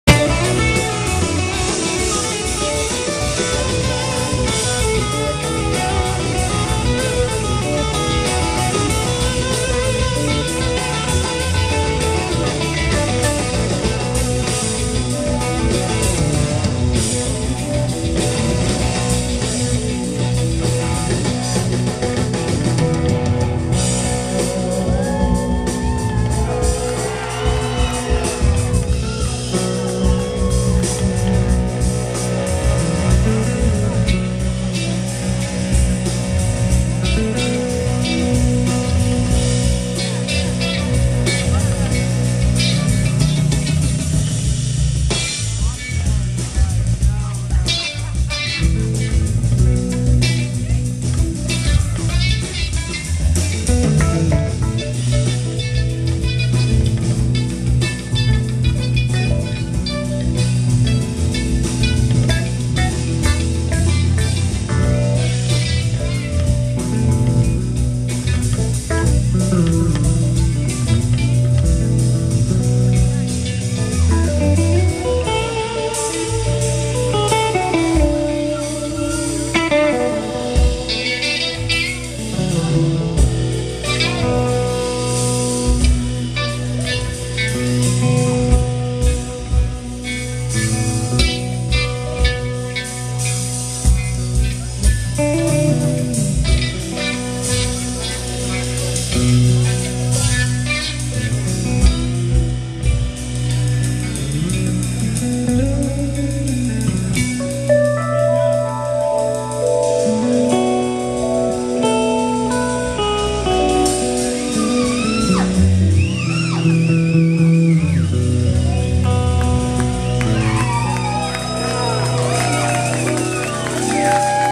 The Chris Robinson Brotherhood @ The Echoplex Los Angeles CA 4-11-11
Chris Robinson - Vocals, guitar
Neal Casal - Guitar
Adam MacDougall - Keyboards
George Sluppick - Drums
Mark "Muddy" Dutton - Bass